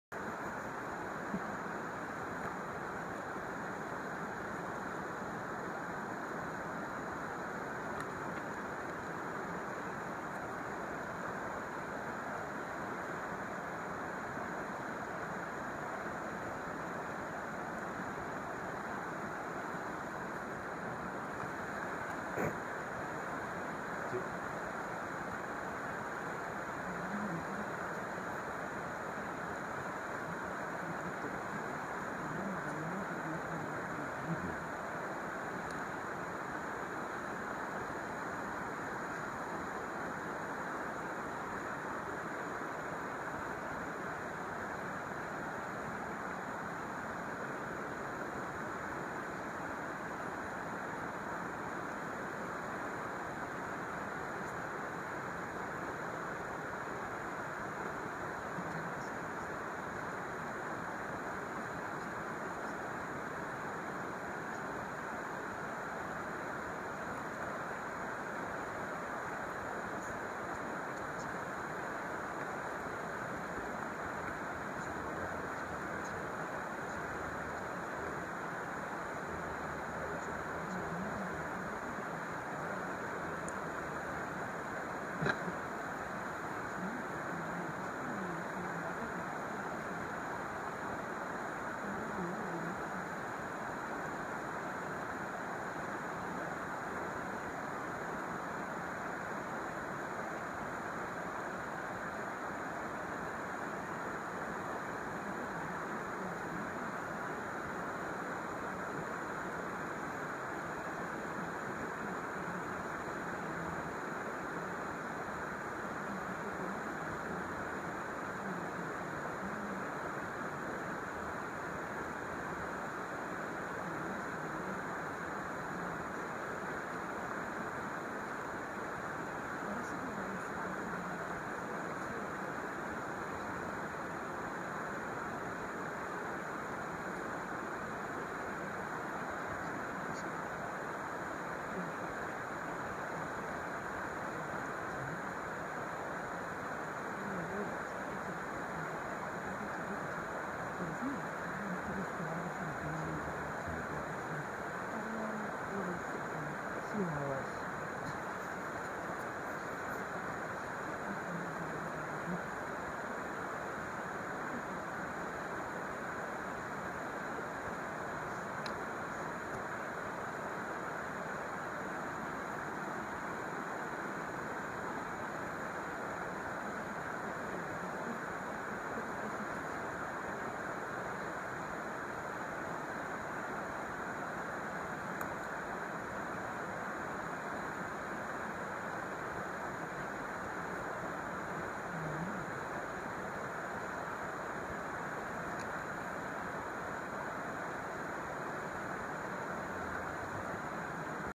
Dipper, Somontano
The county of Somontano hosted Nature Travel Network for three nights in March, 2014. This Dipper was an unexpected and enjoyable find.